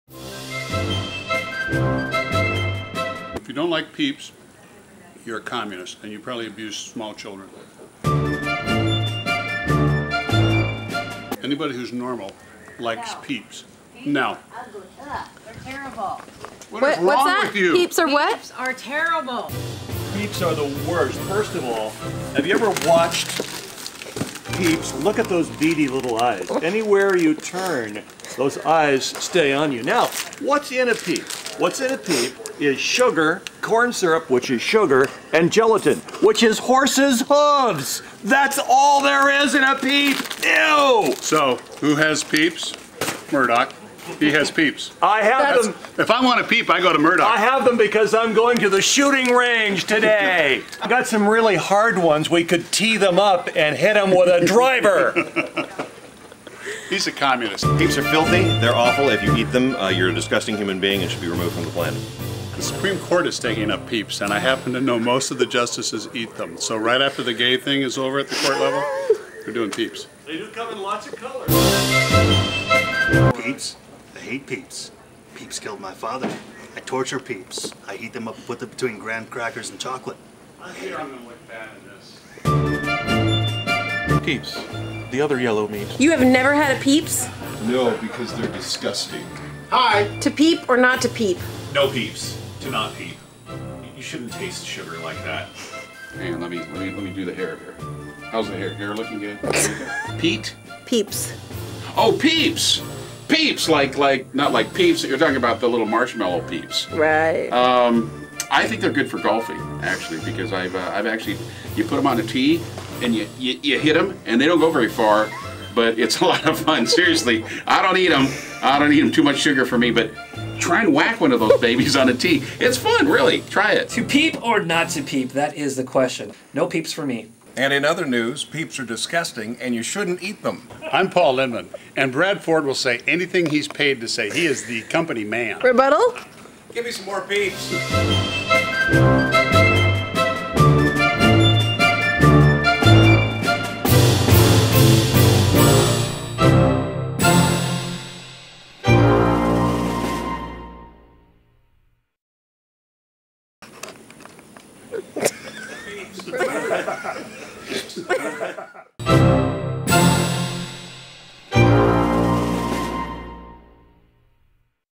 The Marshmallow Peep: To Peep or Not To Peep
Paul Linnman from KEX and Bruce Murdock from K103 have a spirited and "rational" debate about the classic easter treat, Marshmallow Peeps.  Then a bunch of stars from some of your other favorite stations jump into the fray...
A VERY special thanks to SLEDGEHAMMERSMASH on YouTube for the amazing Peep ending!